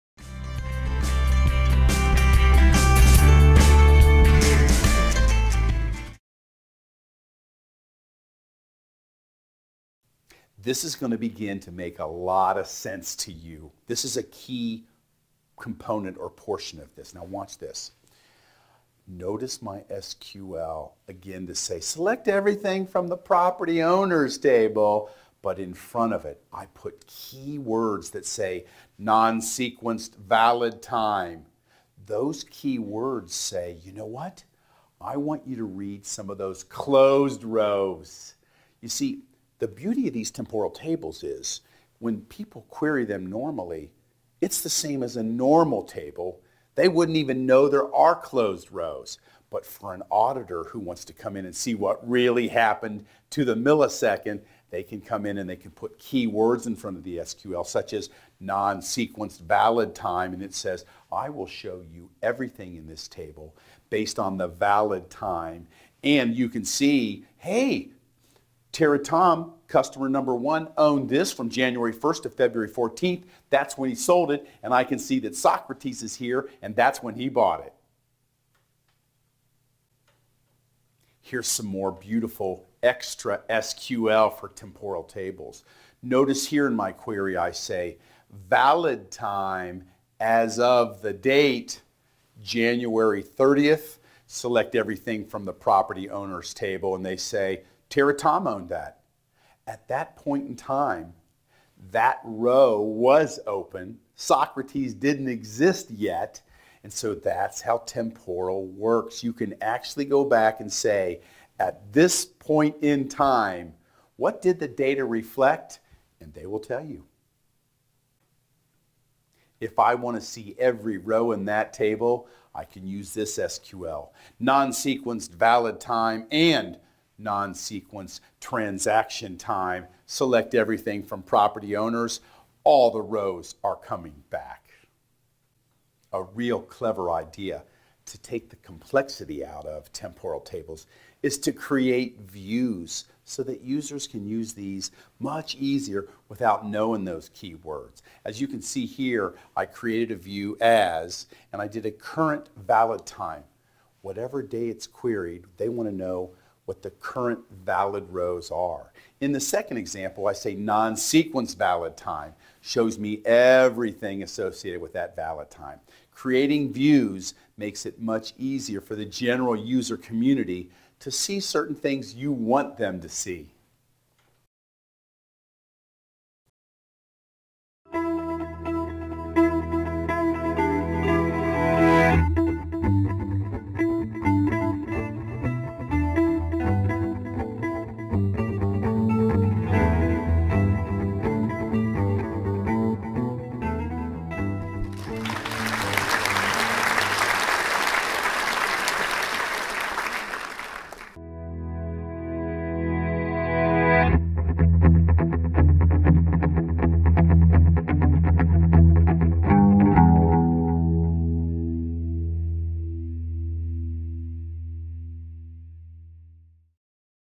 Let's Learn Teradata Lesson 107: Non-Sequenced SQL in Temporal Tables
Explore Teradata with Tera-Tom of Coffing Data Warehousing! In this lesson, le